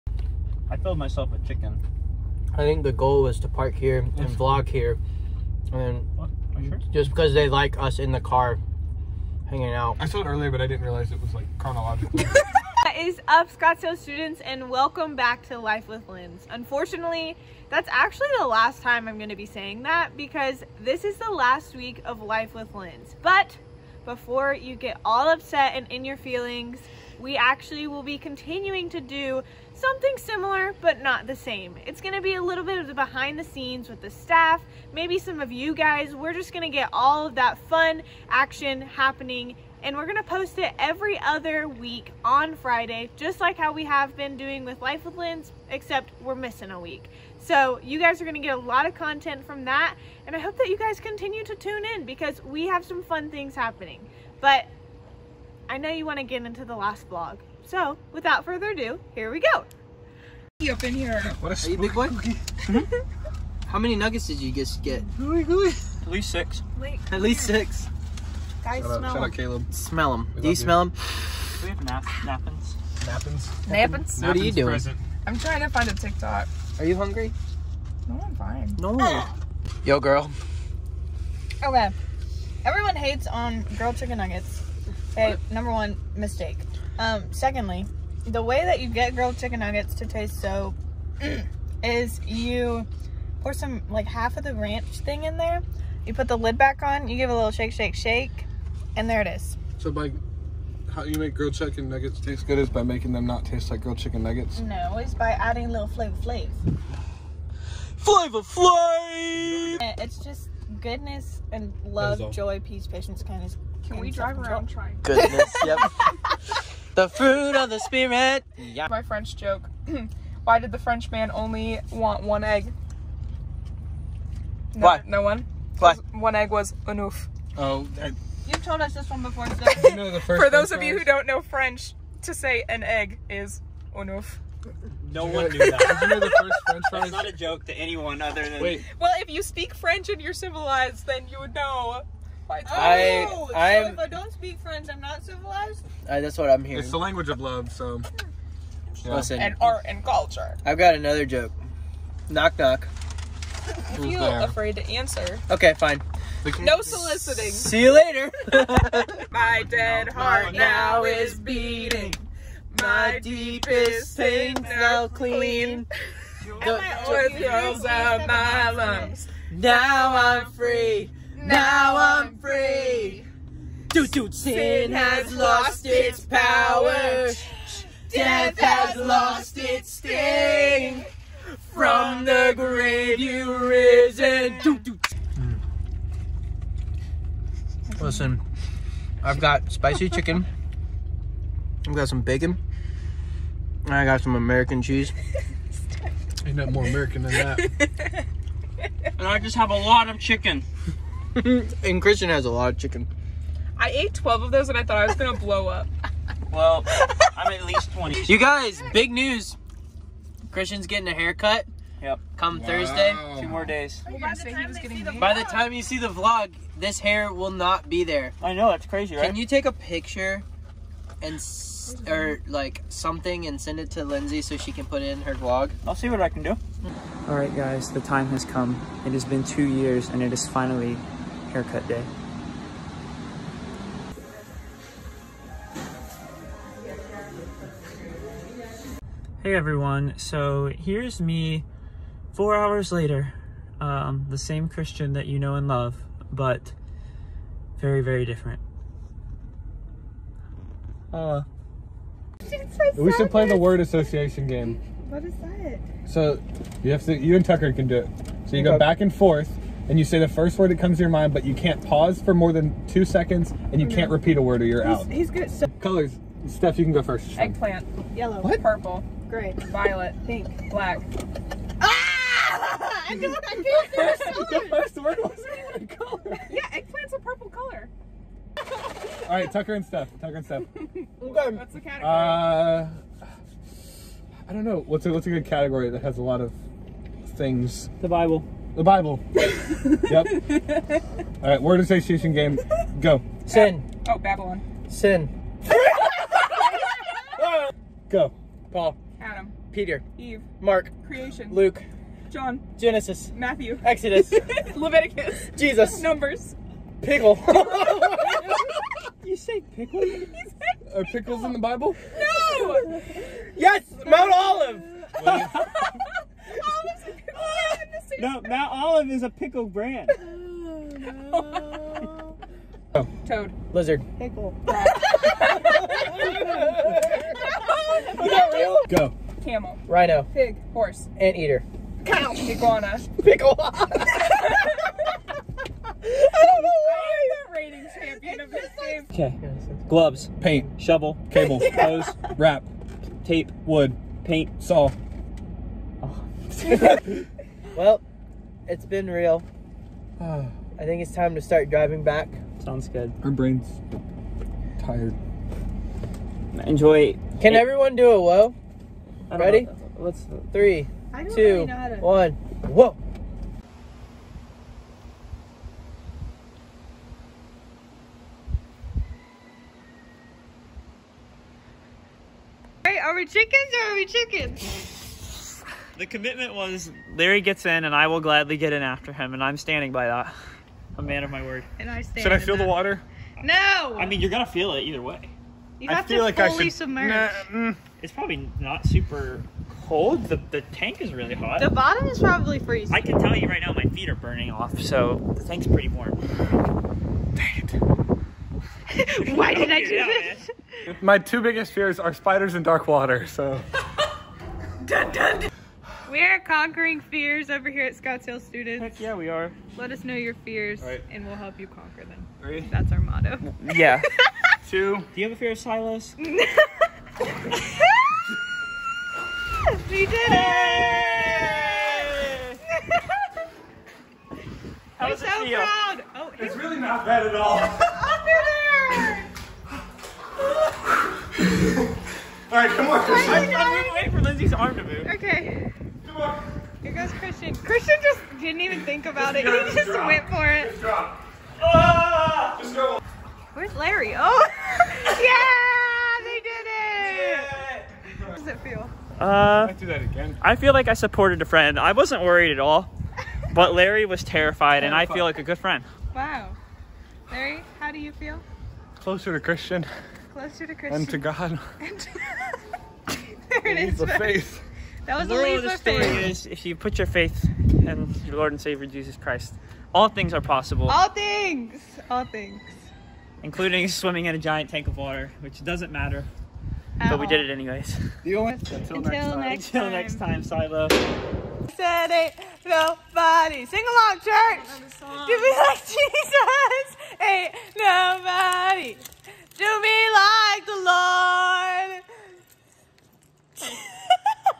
Life with Linds // LAST VLOG
This is the LAST vlog with Linds! We will be starting a BRAND NEW set of videos that will be behind the scenes of our ministry and you will get to see some just of the FUN moments! Those will happen every other Friday! We love our students and, AS ALWAYS, you matter!